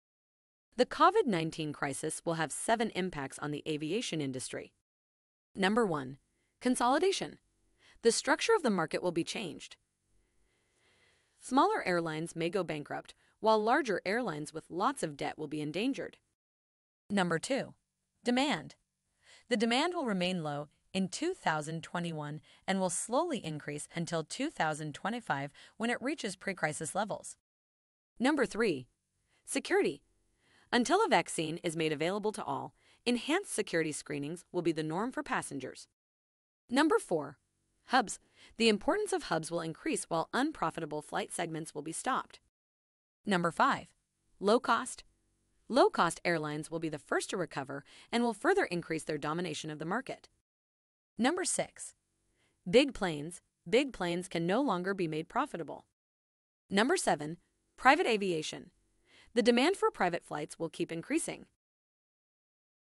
7 impacts of Covid on the airlines industry
Learn more about the 7 impacts of the Covid-19 crisis on the airlines industry
1) Consolidation
2) Demand
3) Security
4) Hubs
5) Low-cost
6) Big planes
7) Private aviation

FULL TRANSCRIPT OF THE VIDEO

The Covid19 crisis will have 7 impacts on the aviation industry.

Number one: consolidation. The structure of the market will be changed. Smaller airlines may go bankrupt, while larger airlines with lots of debt will be endangered.

Number two: demand. The demand will remain low in 2021 and will slowly increase until 2024 or 2025 when it reaches pre-crisis levels.

Number three: security. Until a vaccine is made available to all, enhanced security screenings will be the norm for passengers.

Number four: hubs. The importance of hubs will increase while unprofitable sections will be stopped.

Number five: low-cost. Low-cost airlines will be the first to recover and will further increase their domination of the market.

Number six: big planes. Big planes can no longer be made profitable.

Number seven: private aviation. The demand for private flights will keep increasing.